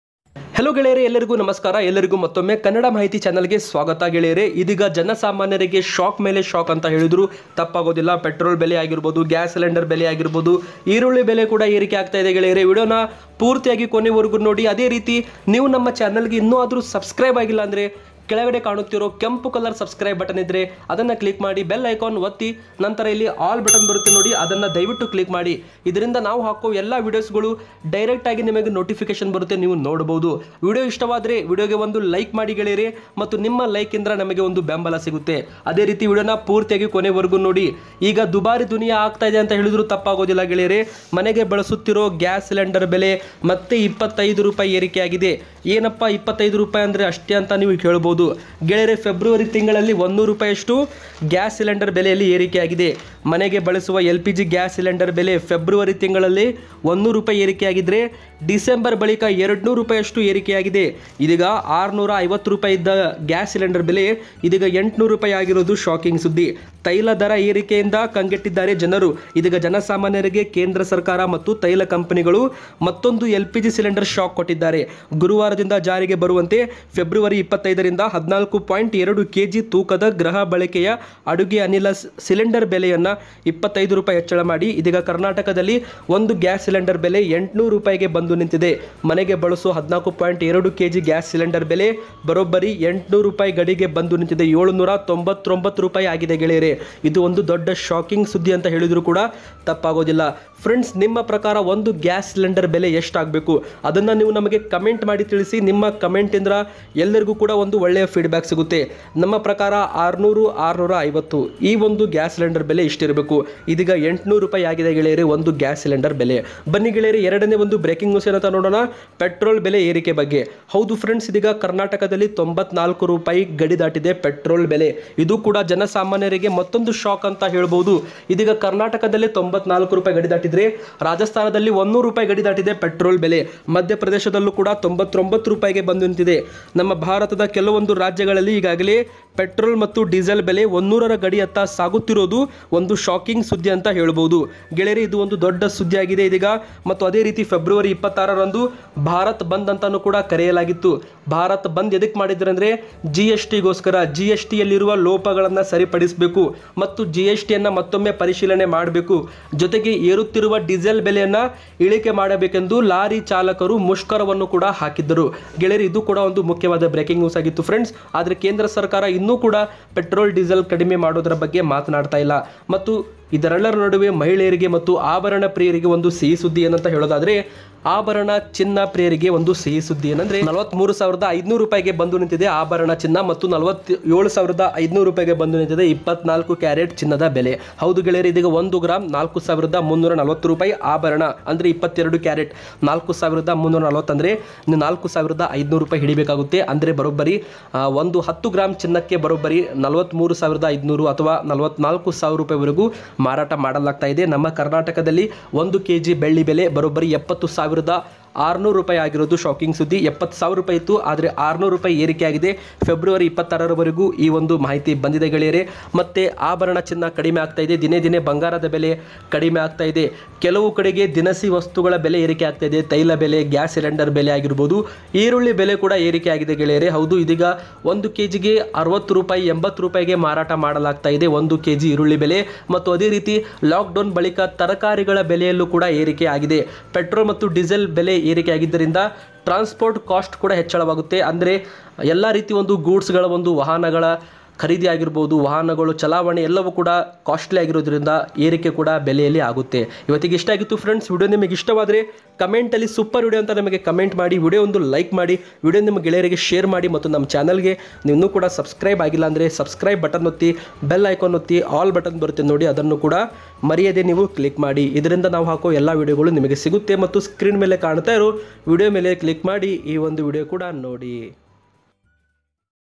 ನಾಳೆ 27 ಫೆಬ್ರುವರಿ : Gyas cylinder : ಗ್ಯಾಸ್ ಸಿಲಿಂಡರ್ ಇದೀಗ 200 ರಷ್ಟು ಏರಿಕೆ! ದೊಡ್ಡ ಶಾಕ್ ಪೆಟ್ರೋಲ್ ಏರಿಕೆ
Gyas Cylinder
petrol
gold rate
news